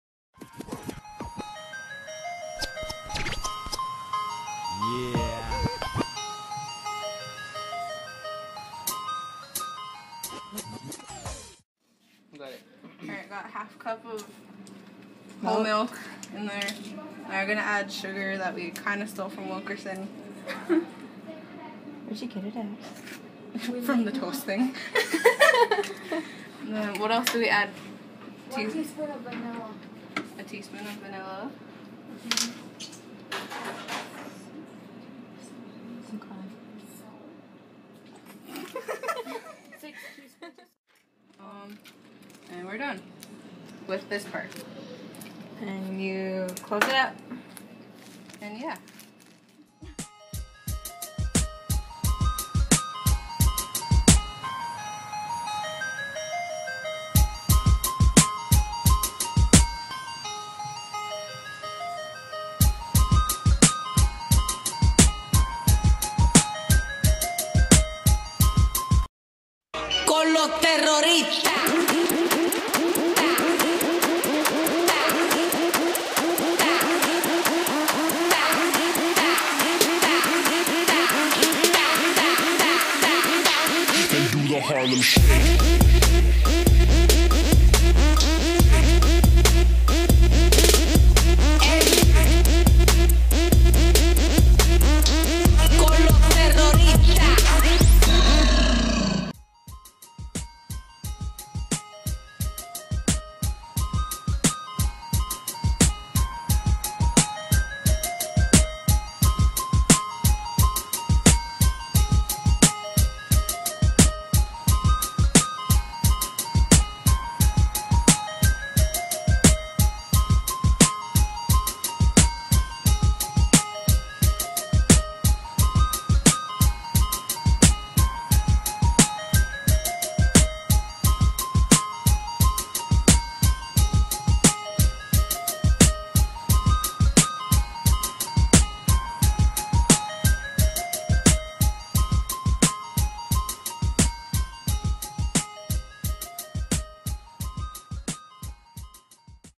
Ice Cream Making
A video that me and my team did for a group presentation at INMED 2013